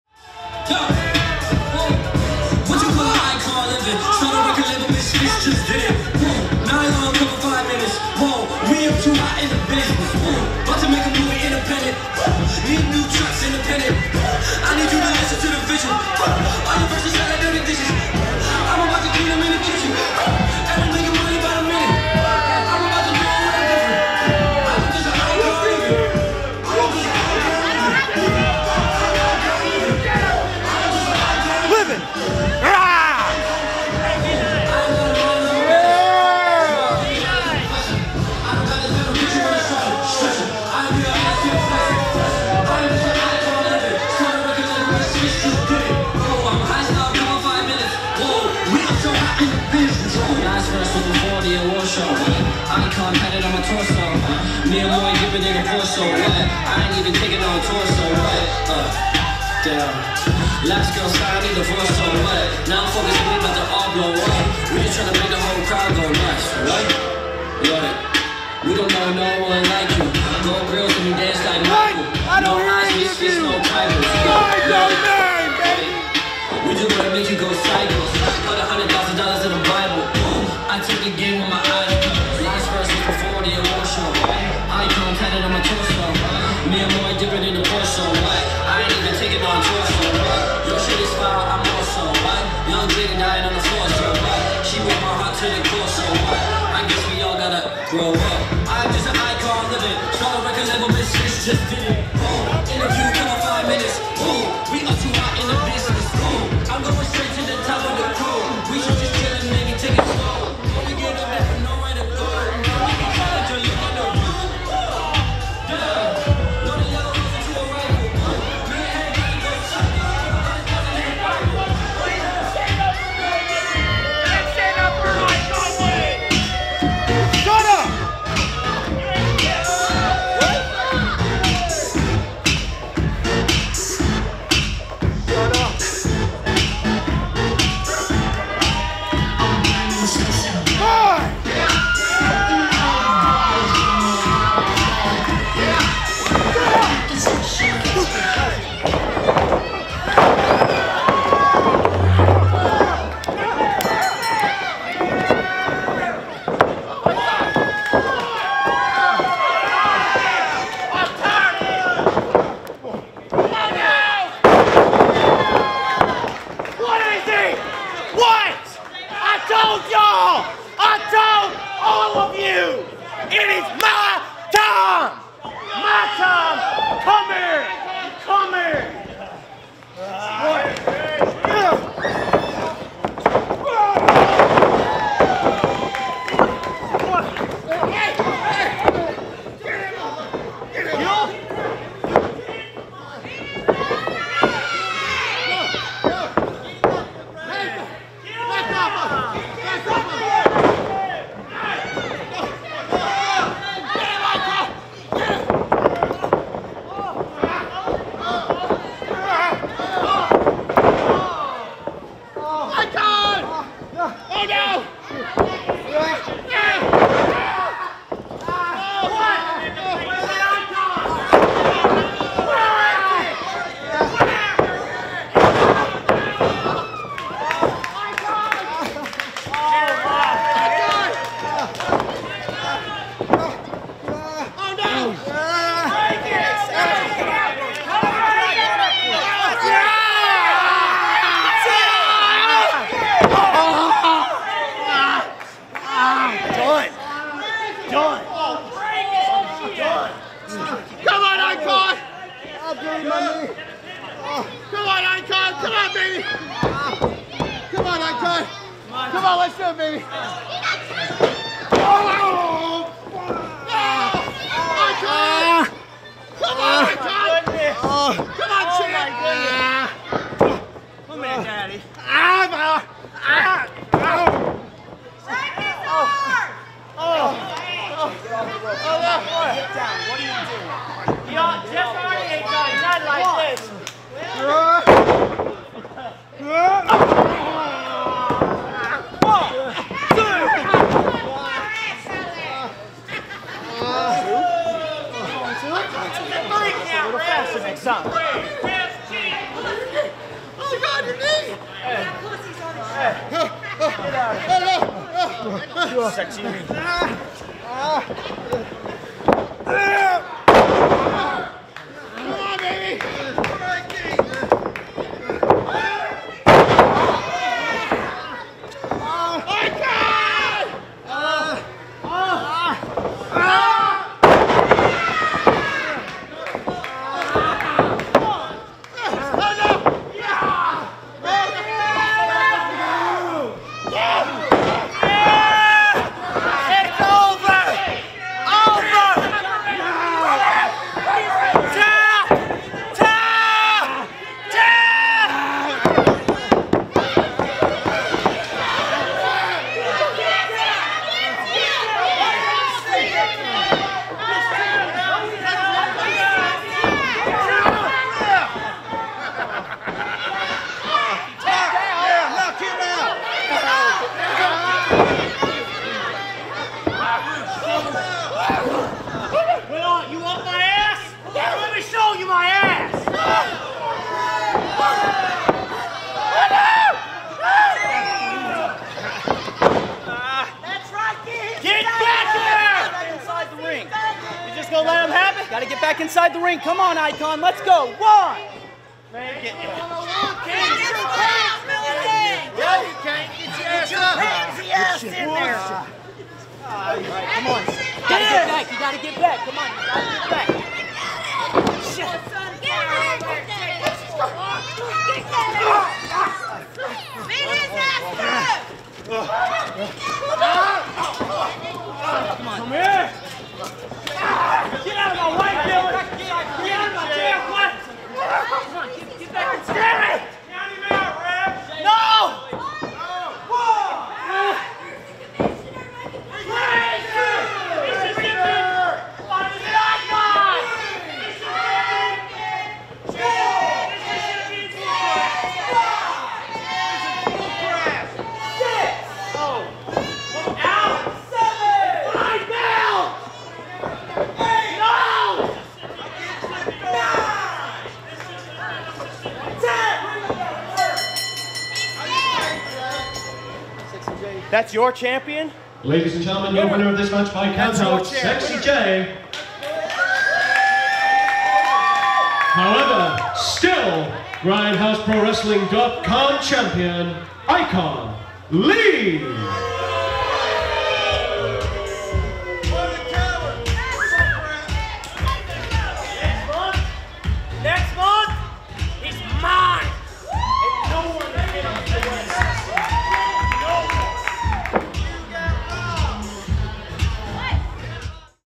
ICON vs SEXY J
ICON vs SEXY J! After Crying & Demanding I cave & give Sexy J his rematch! To bad Im always going to be a Smart Champion! Filmed by Hector & Grind House Pro Wrestling.